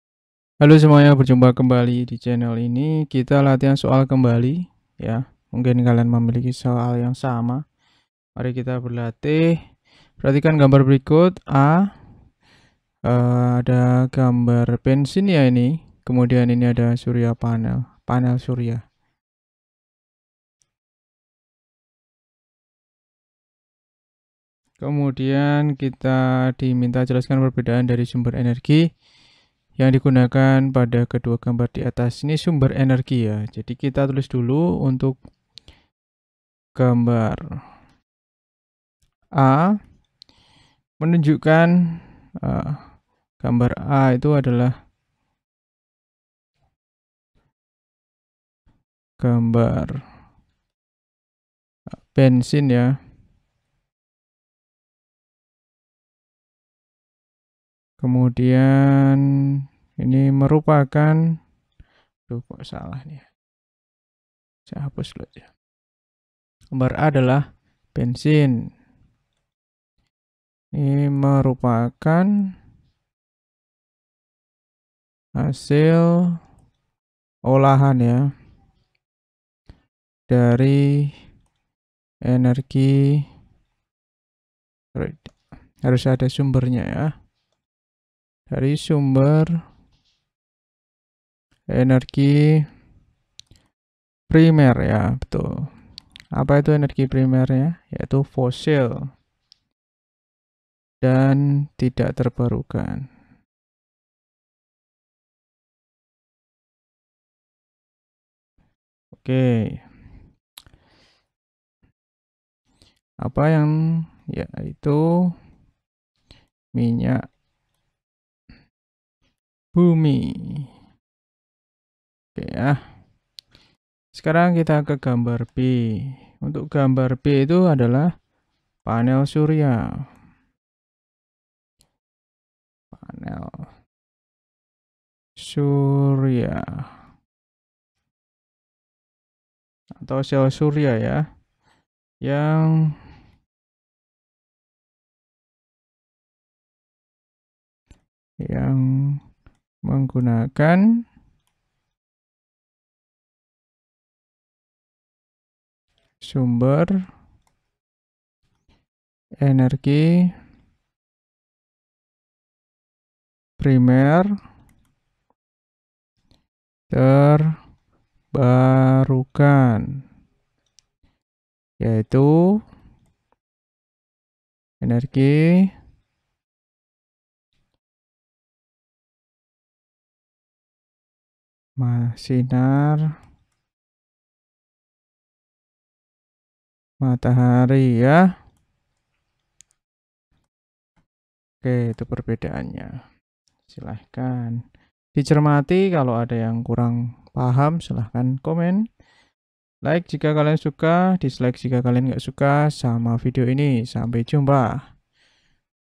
Perhatikan gambar berikut.
Jelaskan perbedaan dari sumber energi yang digunakan pada kedua gambar di atas